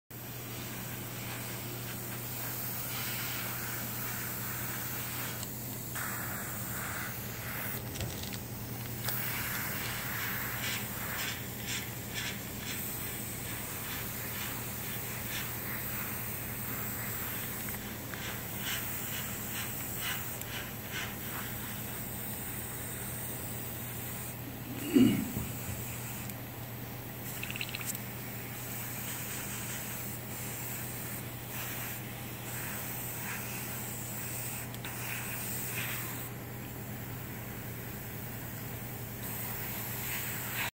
튜닝타임즈 프라모델 강좌 천호팀 김윤준님의 Italery 이탈레리 1/35 M8 Grey Hound 그레이하운드 에어브러시 색칠
현대백화점 천호점 문화센터 프라모델 에어브러시 수업 수강생 김윤준님이 에어브러시를 사용하여 장갑차 외관과 내부 인테리어를 칠해주고 있는데요, 작업하는 중간중간 에나멜 도료를 붓에 찍어 안쪽 잘잘한 소품을 칠하면서 진행하고 있습니다^^